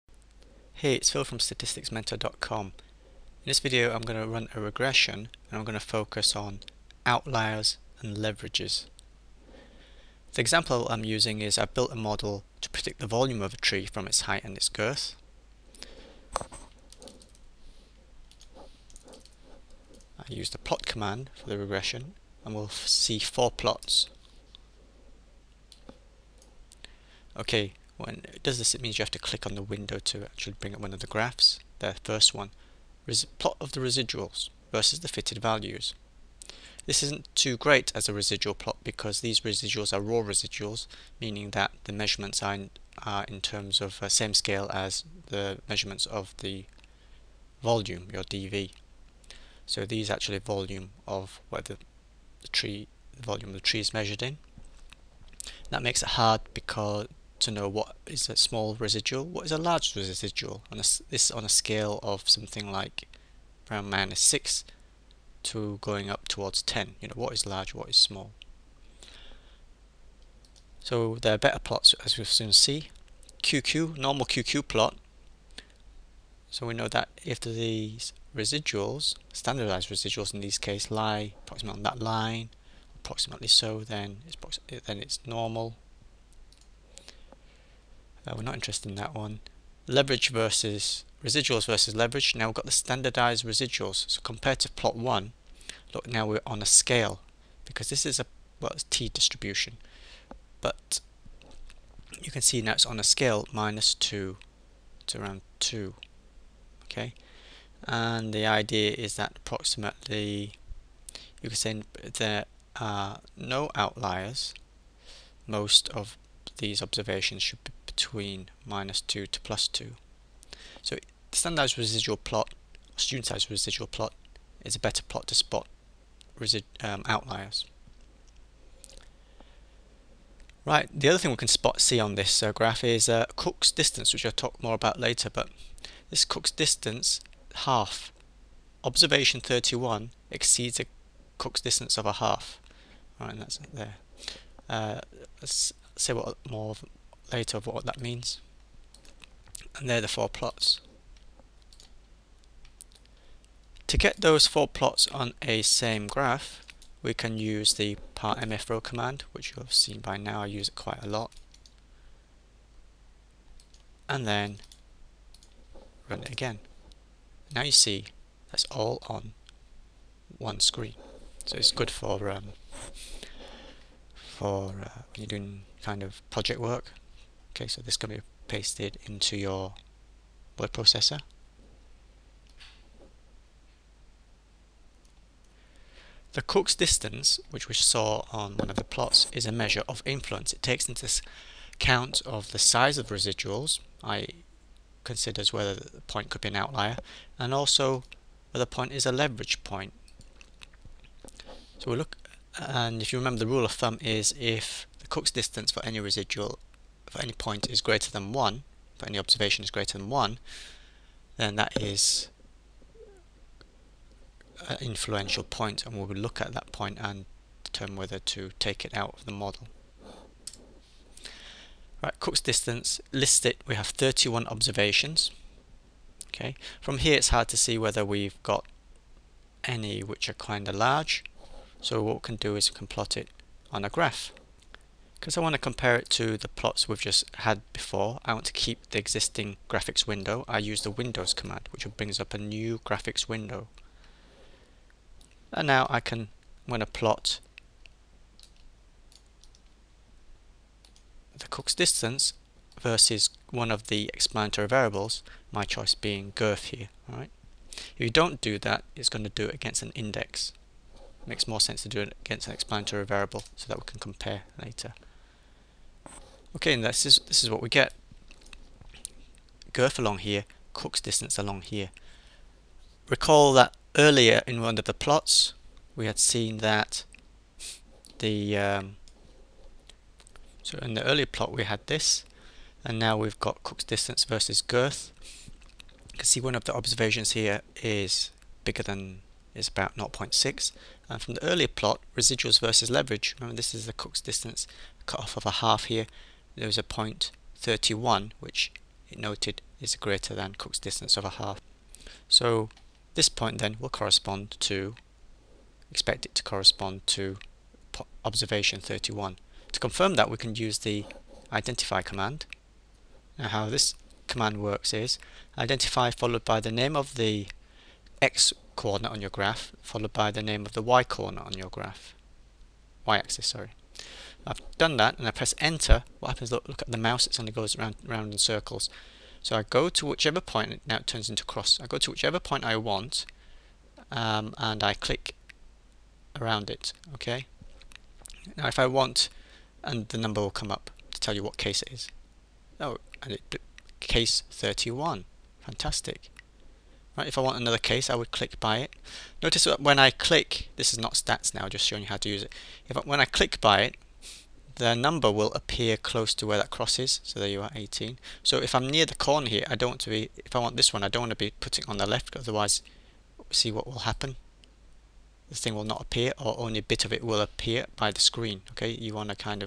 Statistics with R: Example of outlier and leverage analysis part 1 of 3
I show you how to go about determining whether there are outliers and points of high leverage in your model. For outliers we can look at the plot of the standardized or studentized residuals. For points of high leverage we can look at a plot of the leverage values, and we can use Cook's distance as a measure of the combined influence of size of (standarddized) residual and size of leverage.
Along the way we will make use of nice commands in R that do some smart things :)